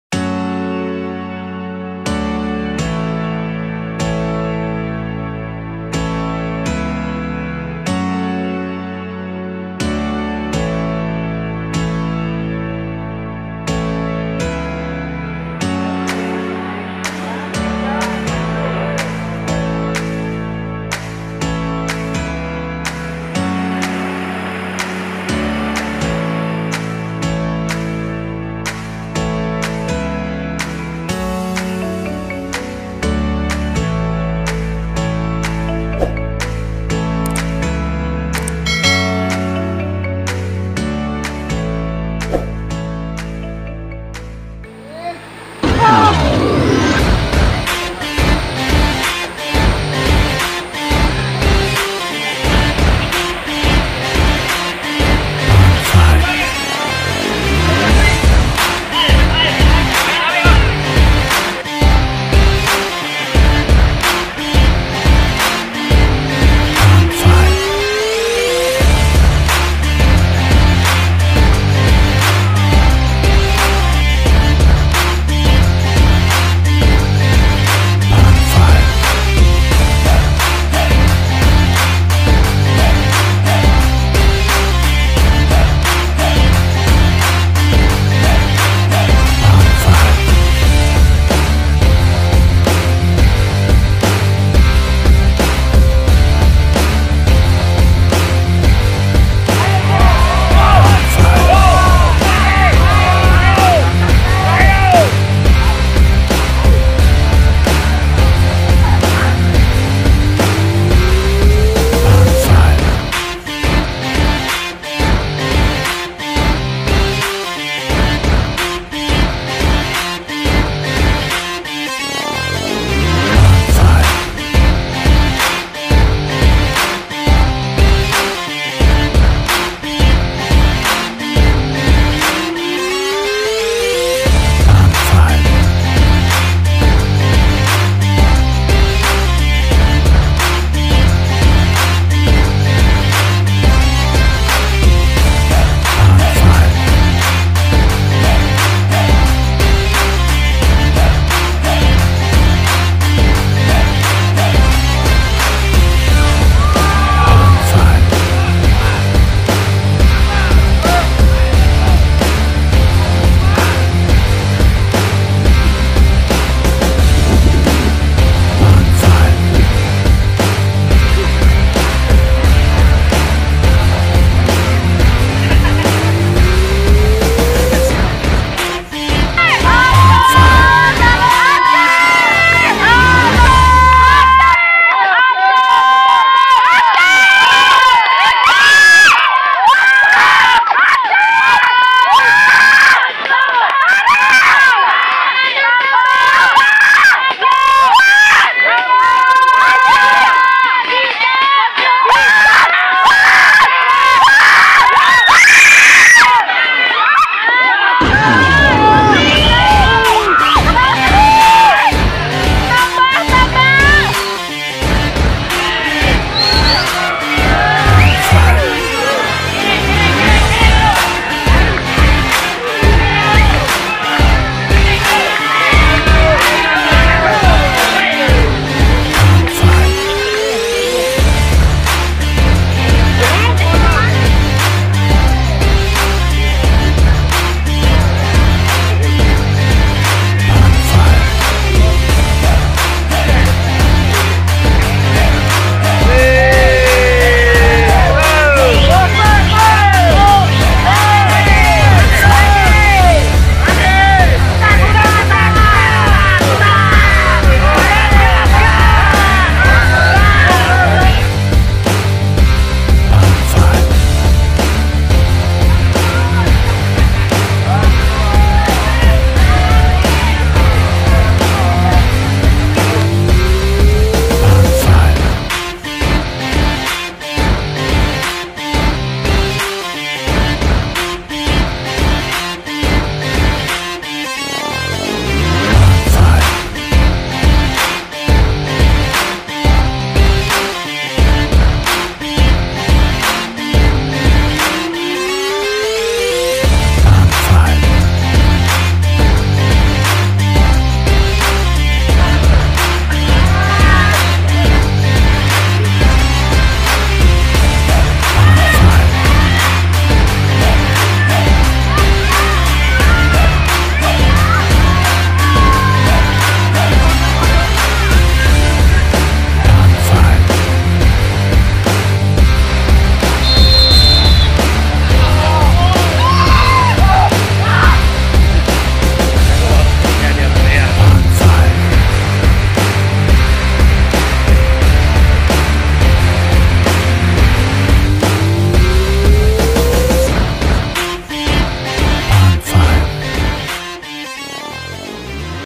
ARUNG JERAM //Kejuaraan NASIONAL Hari Ke Tiga - PWK NEWS
Kejuaraan Nasional Arung Jeram di way Besai hari ke tiga.
team PWK NEWS